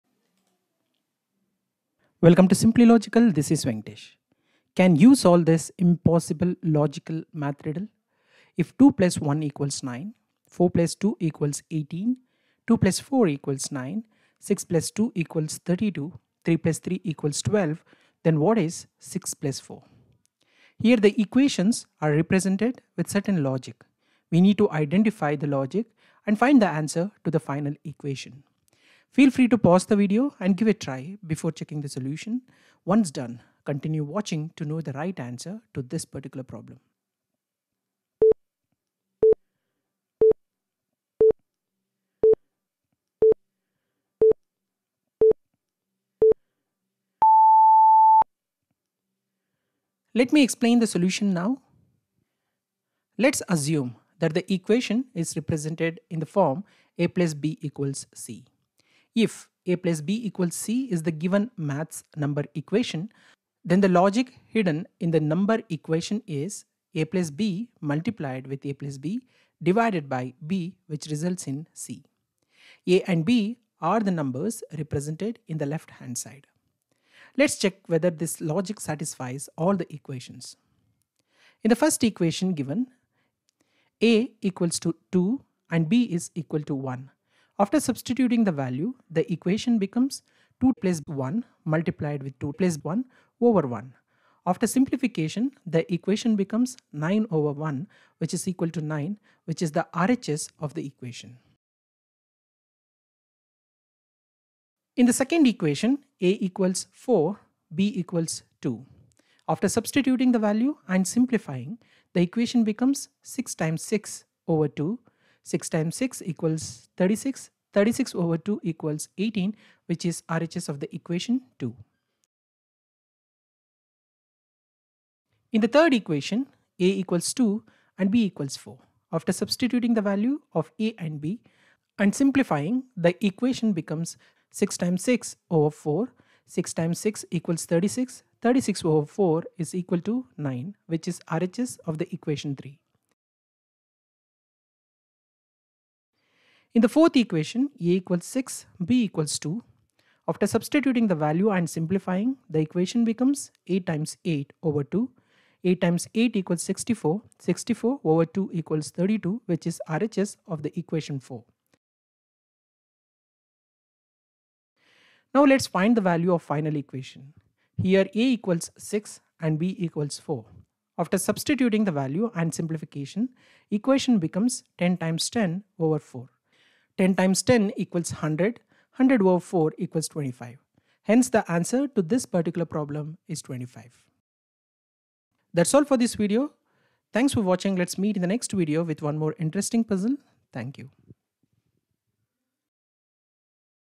Impossible Math Riddle || Find The Missing Number Puzzle
Can you solve this impossible logical Math riddle
if
2 + 1 = 9
4 + 2 = 18
2 + 4 = 9
6 + 2 = 32
3 + 3 = 12
then
what is 6 + 4
Here the equations are represented with certain logic we need  to identify the logic and find the answer to the final equation.